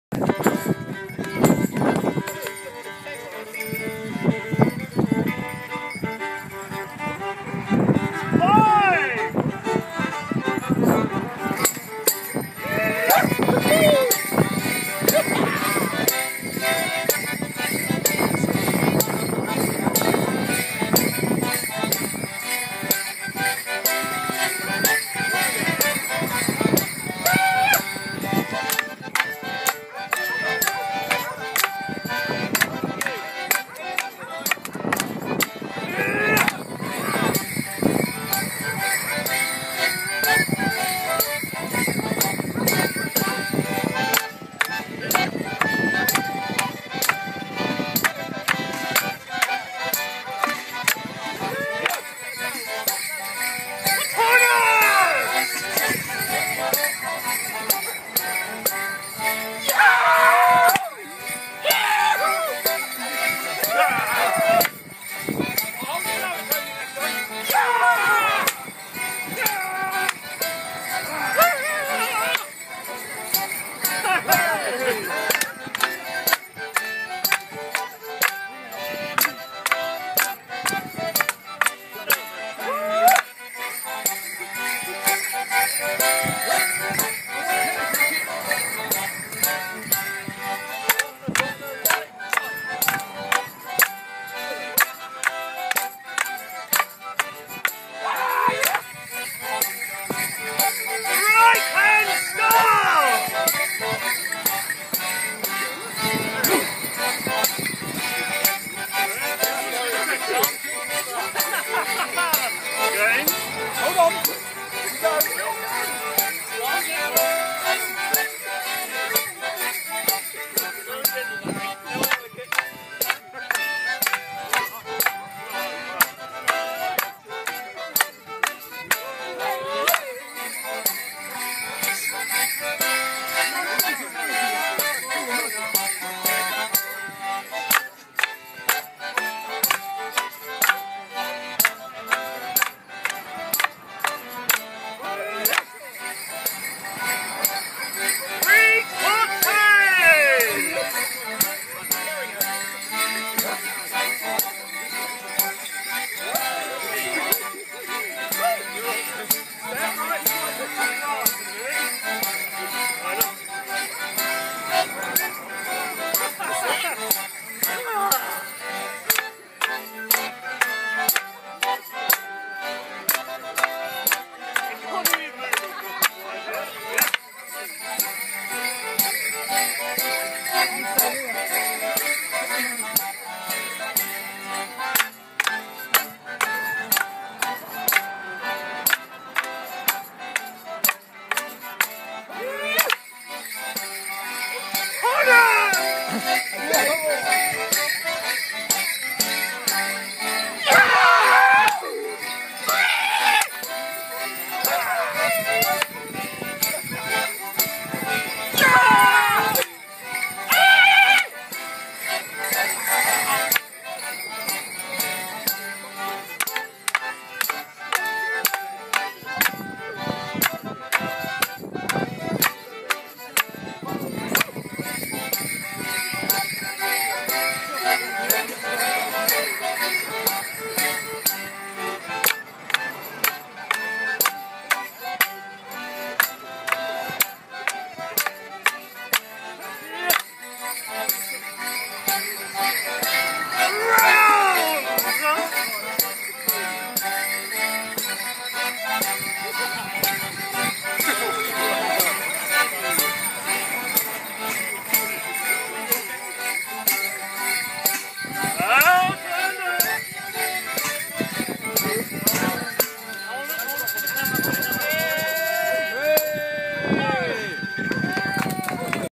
Hook Allotments Wassail Morris Dance 2013
Featuring the Hook Eagle Morris Men